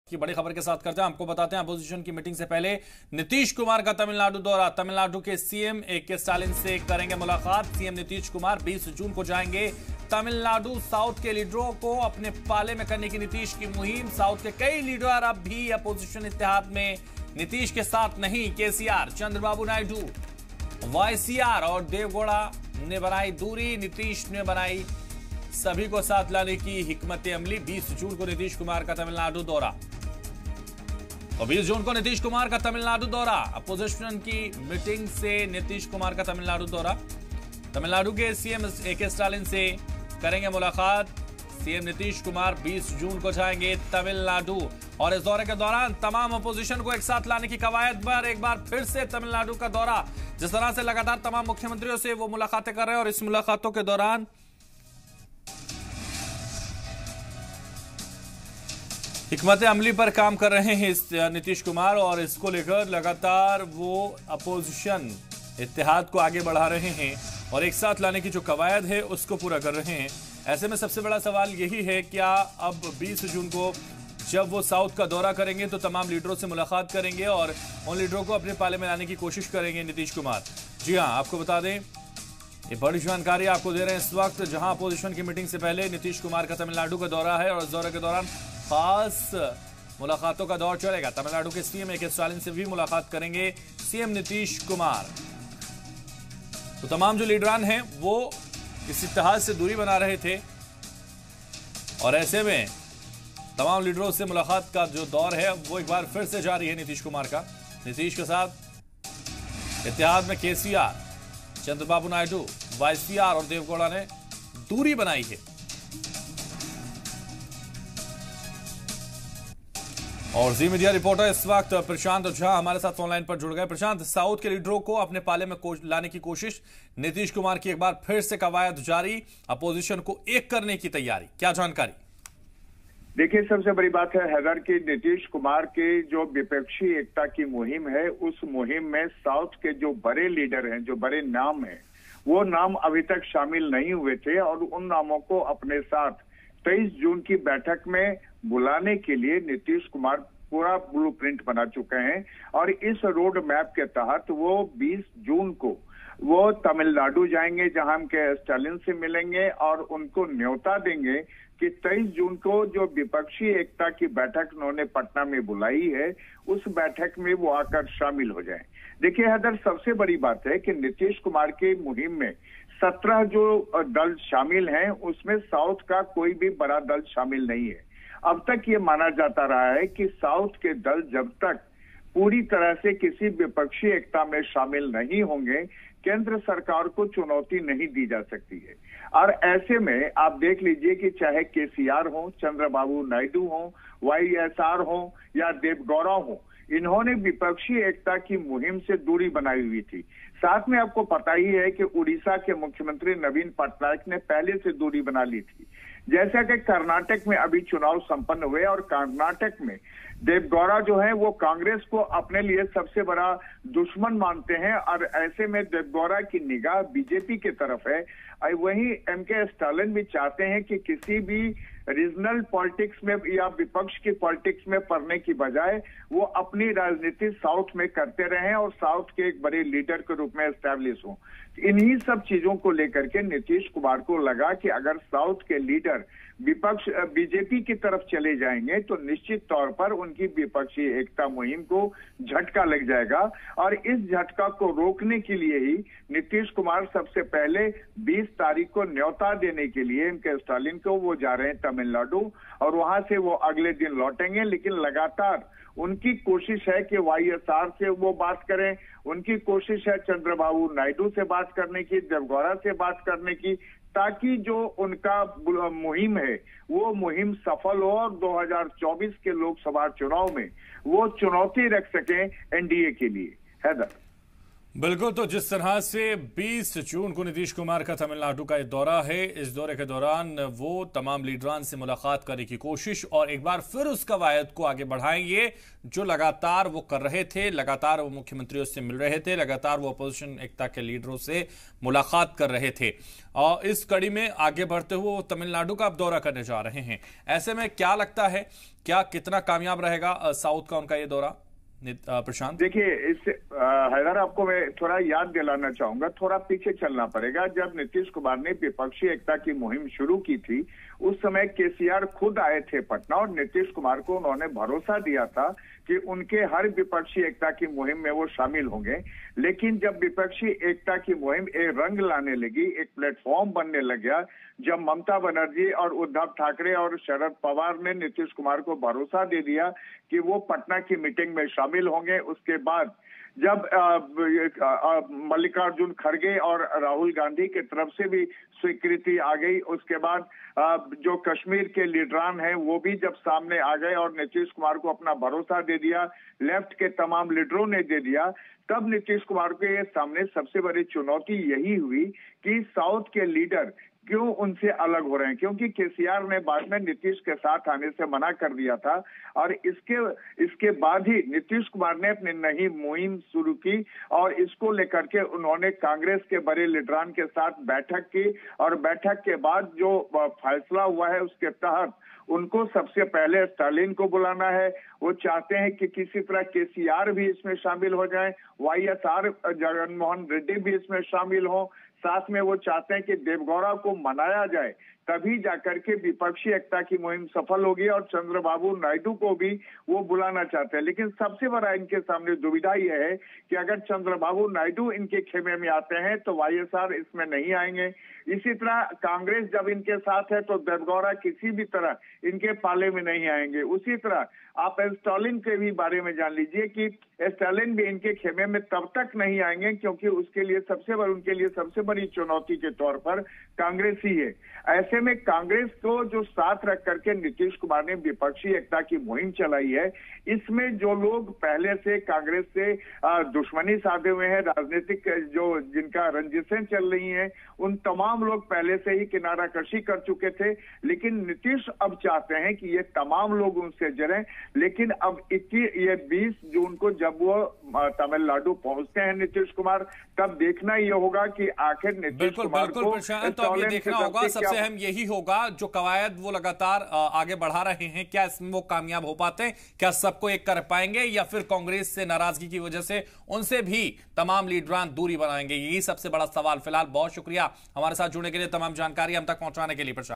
Breaking news: 20 जून को Nitish Kumar का Tamilnadu दौरा | Bihar news | Urdu news | Zee Salaam
Breaking news: 20 जून को Nitish Kumar  का Tamilnadu दौरा

Nitish Kumar's Tamil Nadu tour on June 20